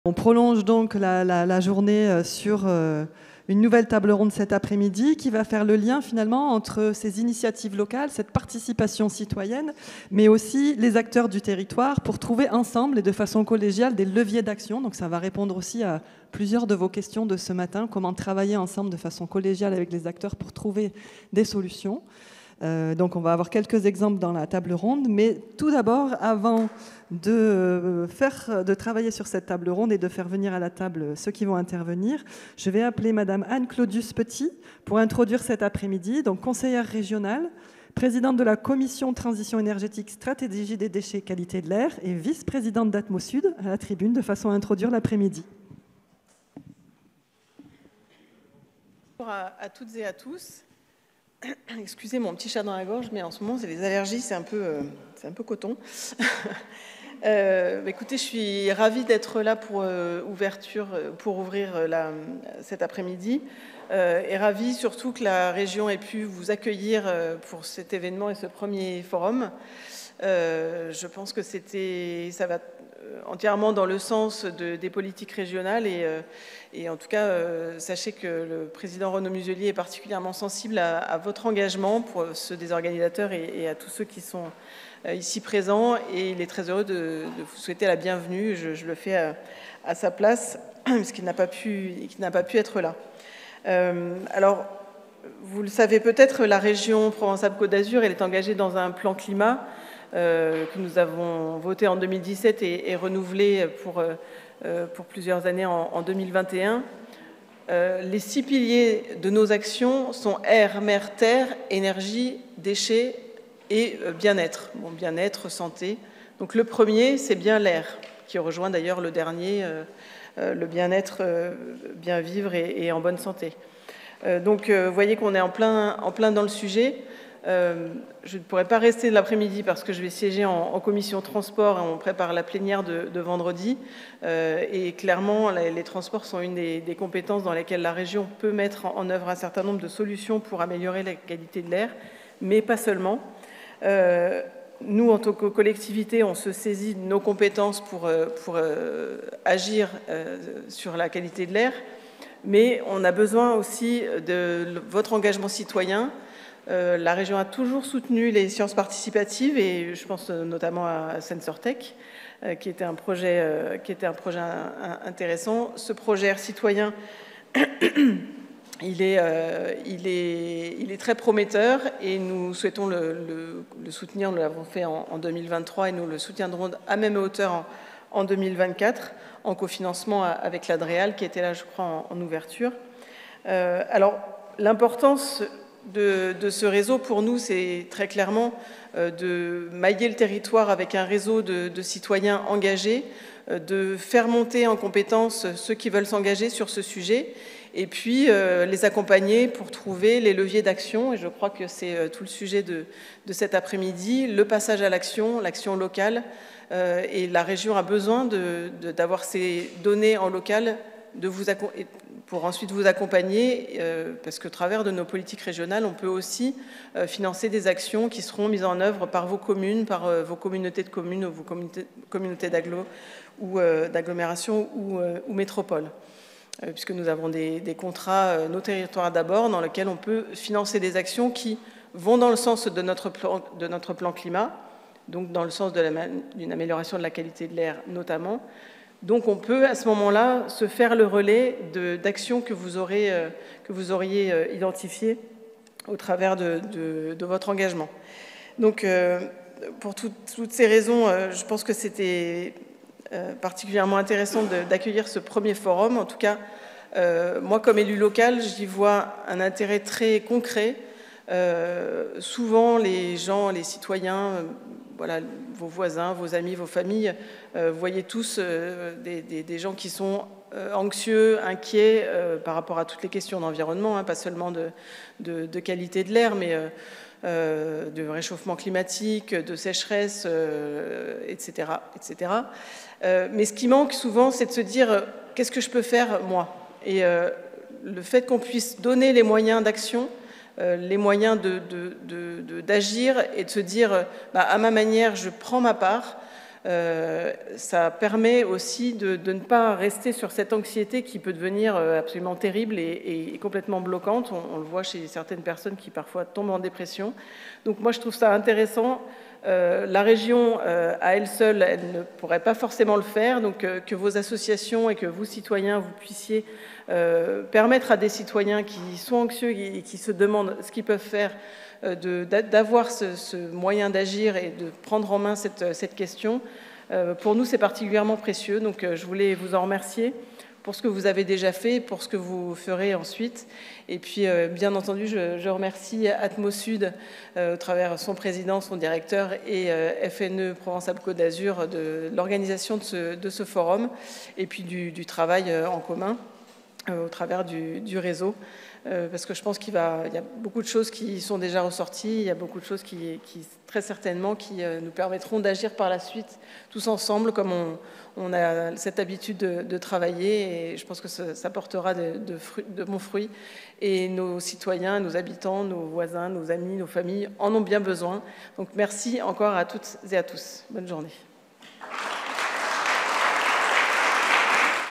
Introduction de l'après-midi du forum citoyen de l'air - 26 mars 2024
Introduction de l’après-midi par Anne CLAUDIUS-PETIT, Conseillère régionale, Présidente de la Commission Transition énergétique, stratégie des déchets, qualité de l'air, Vice-présidente d'AtmoSud